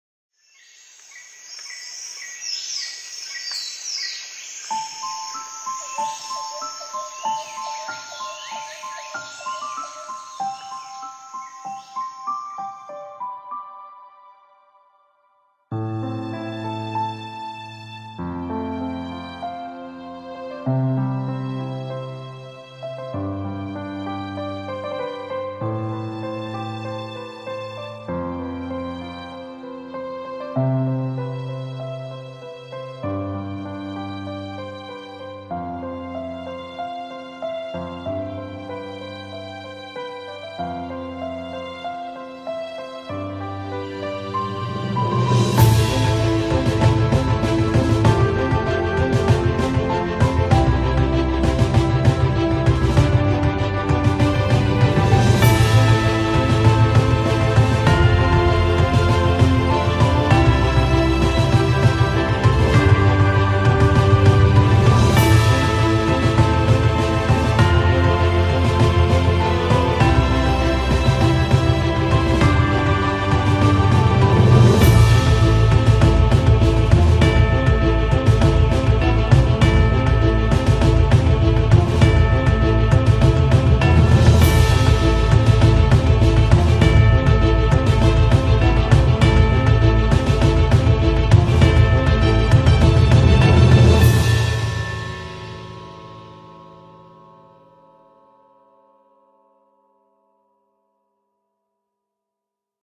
Eco Park, Bandar Seri Botani
Beautiful drone footages taken at Eco Park, Bandar Seri Botani... It's the music that makes everything dramatic. All edited with the iPad Pro using LumaFusion.